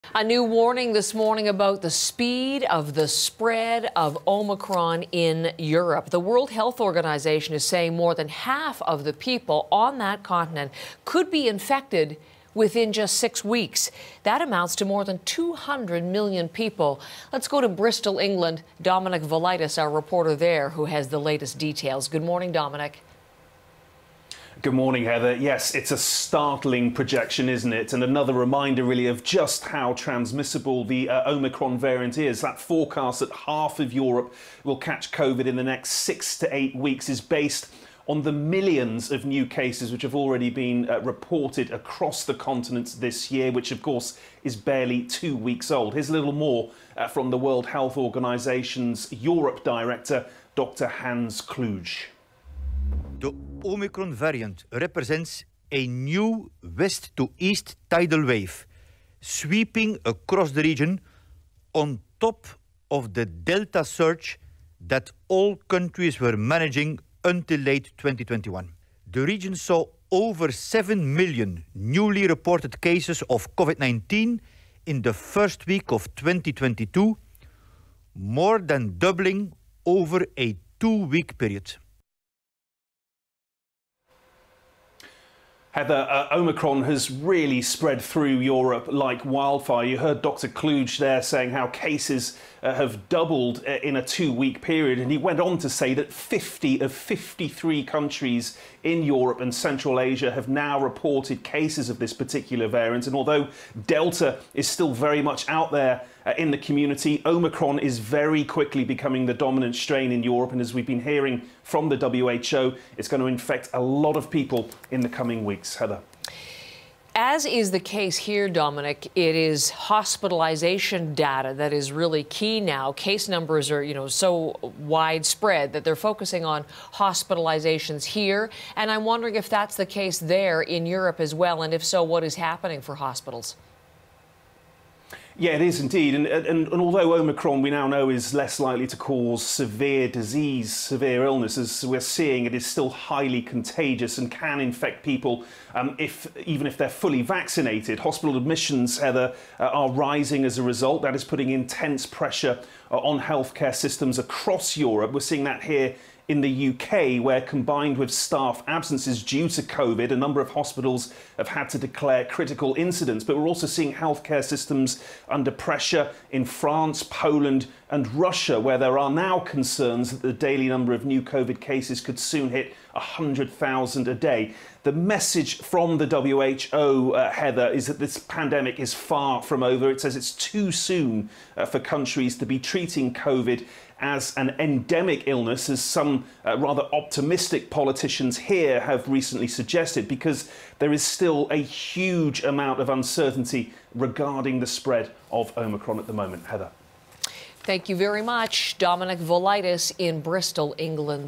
Half of Europe could be infected with Omicron within weeks, WHO says
The World Health Organization says more than 200 million people in Europe could be infected with the Omicron variant of the coronavirus in the next six to eight weeks.